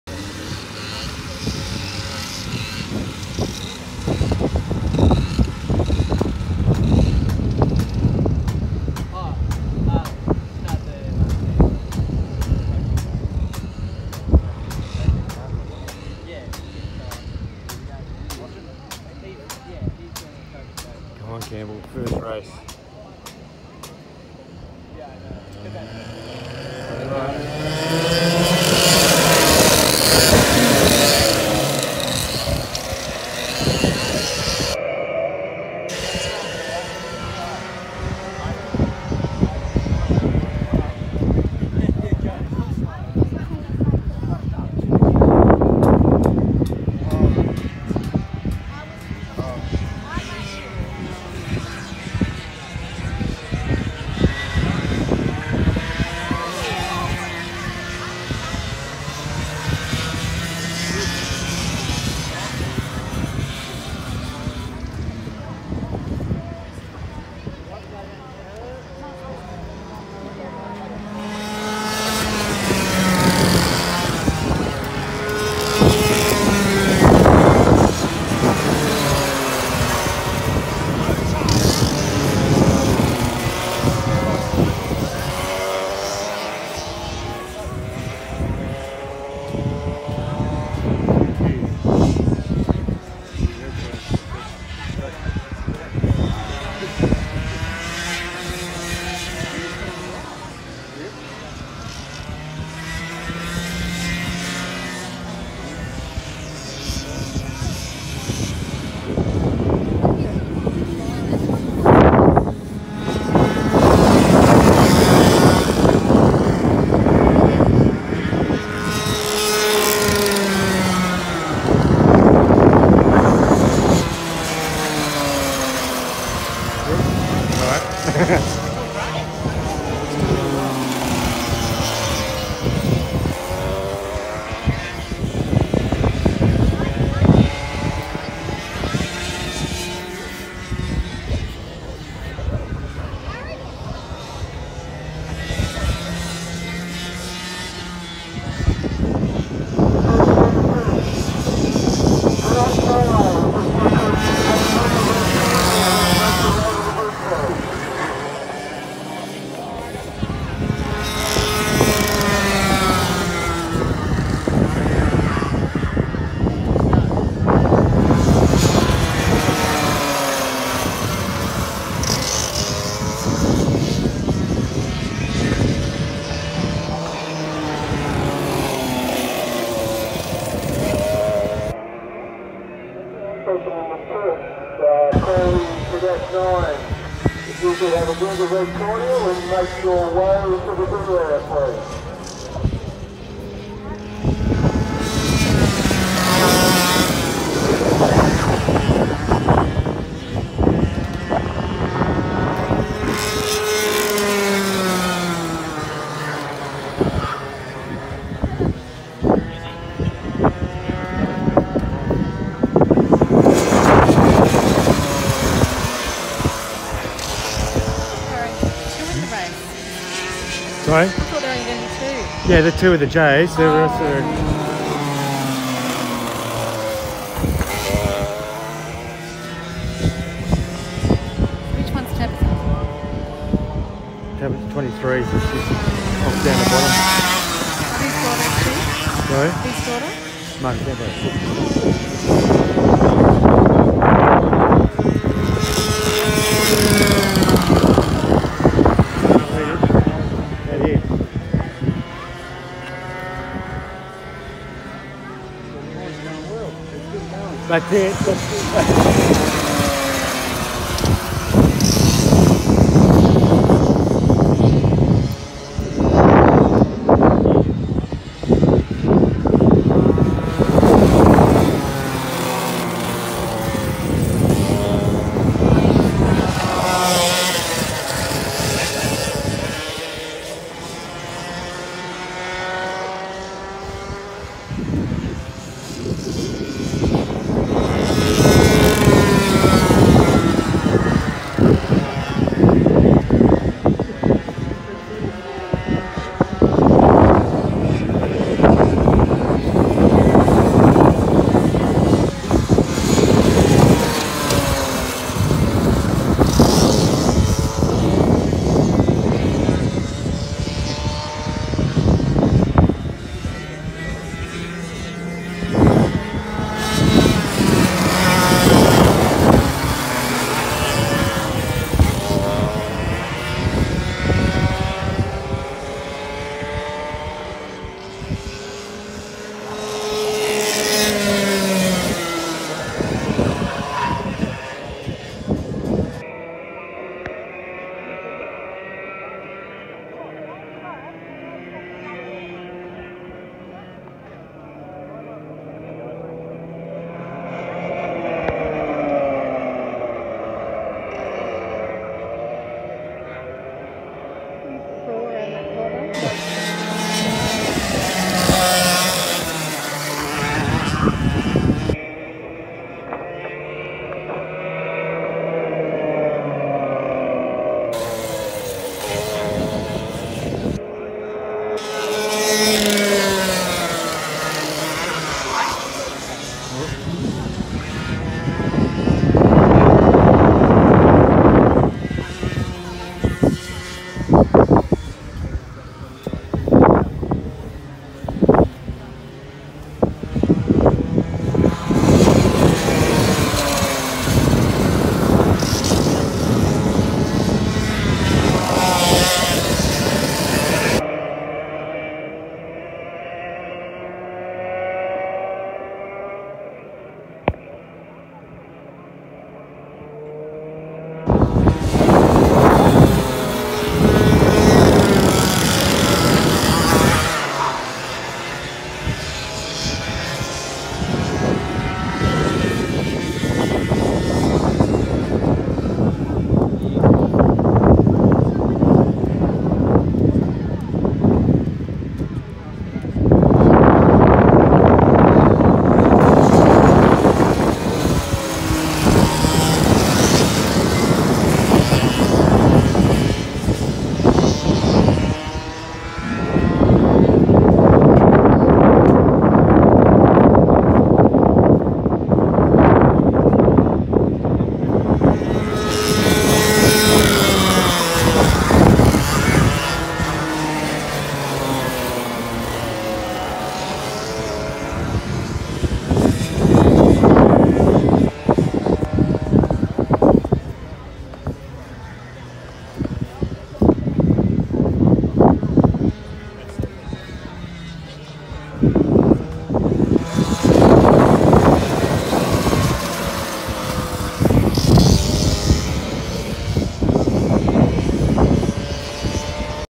Sept 6 2020 My first race
My first race 2 classes in the same field. My friend Charlie and I are in Junior Yamaha. The other 4 were KA4 Junior Heavy.